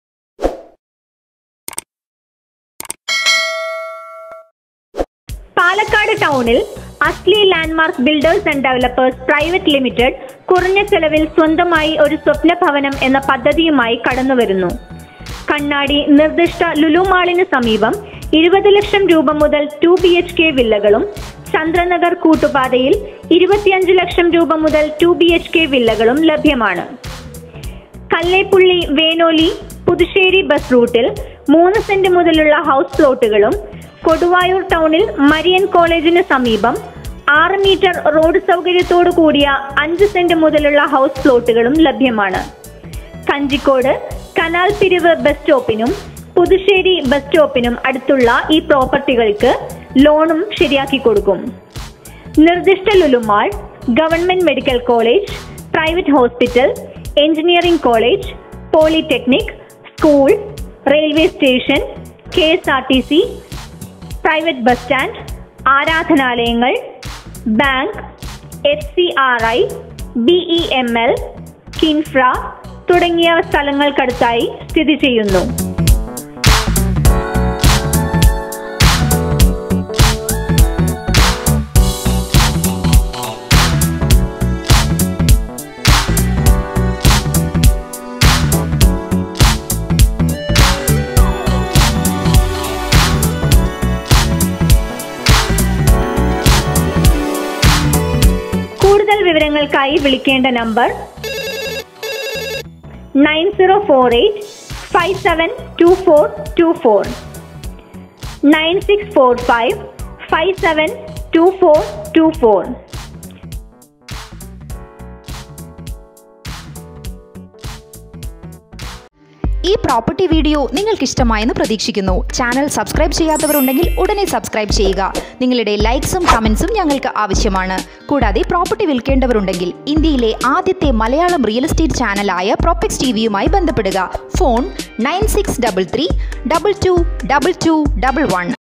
HOUSE FOR SALE IN PALAKKAD|RESIDENTIAL LAND IN PALAKKAD TOWN|narayanankutti plkd 3 02 2023 akshay
Palakkad town ,"Dream Home in low budget" by Asly Land Mark Builders and Developers.residential plots and houses in Palakkad town available in the project.
Kannadi- near proposed Lulumall 2bhk villas Price-20 Lakhs onwards
Chandranagar Koottupatha -2 bhk villas ,price- 25 Lakhs onwards.
Kallleppulli Venoli Puthussery bus route-- 3 cent onwards house plots
Koduvayoor town -near Mariyan college 5 cent onwards house plots with road facility.
Proposed Lulumall,govt.medical college,poly technic,school,railway station,KSRTC,private bus standworships,bank,FCRI,BEML,KINFRA nearby
For more details: